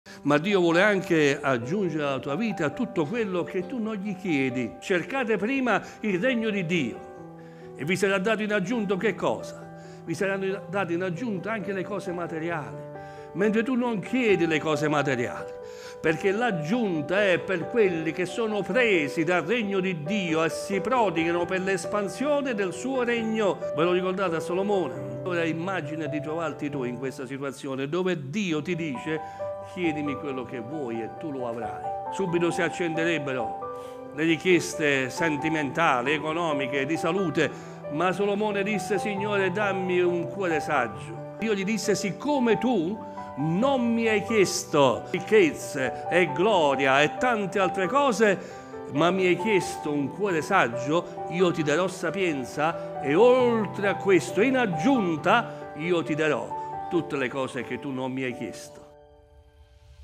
Dio vuole aggiungere alla tua vita tutto quello che non gli chiedi se...
Ascolta tutta la predicazione del Pastore Tino Di Domenico: "l'aggiunta di Dio" sui nostri canali e podcast.